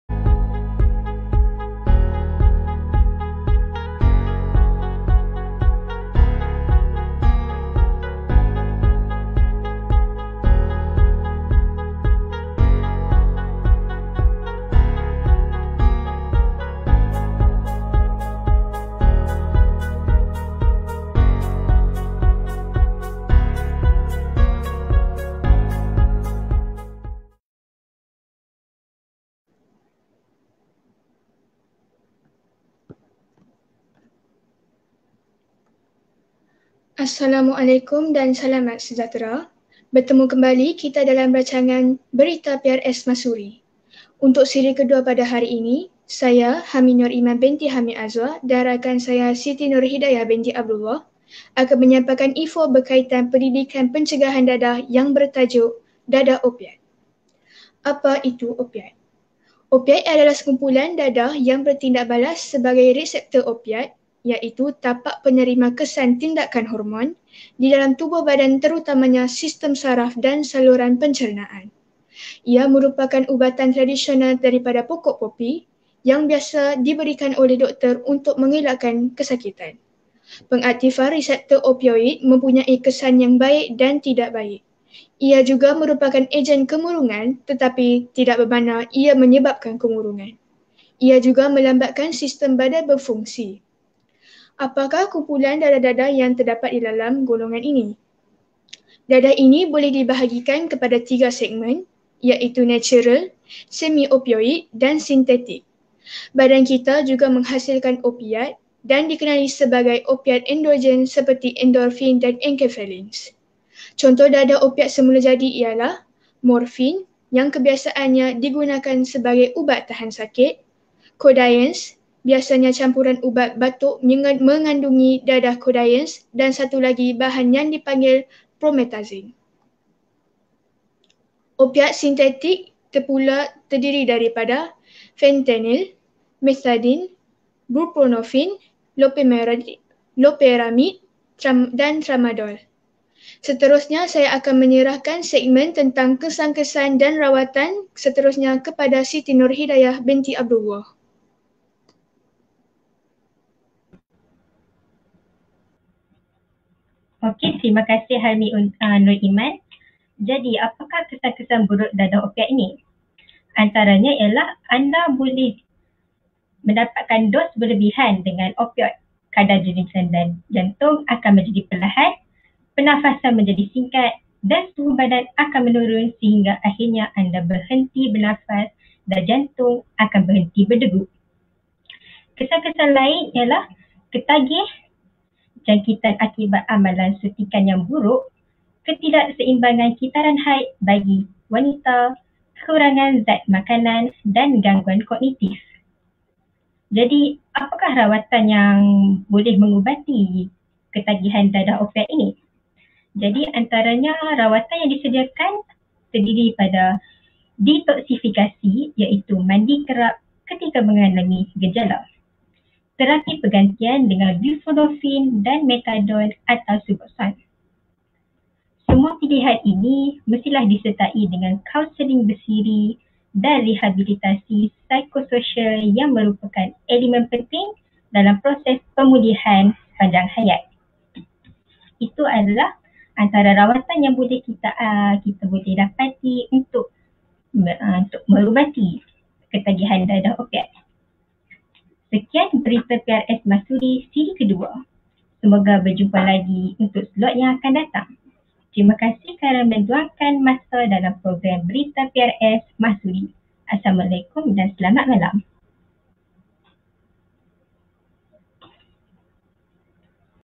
BERITA PRS MAHSURI SIRI 2: DADAH OPIAT
BERITA PRS MAHSURI SIRI 2 dijalankan pada:
26 JUN 2021
SABTU
9.15 MALAM

perkongsian yang bertajuk DADAH OPIAT ini akan disampaikan oleh:
SAUDARI HAMIENURIMAN (4ST) & SAUDARI HIDAYAH (4SK)

Mari dengarkan...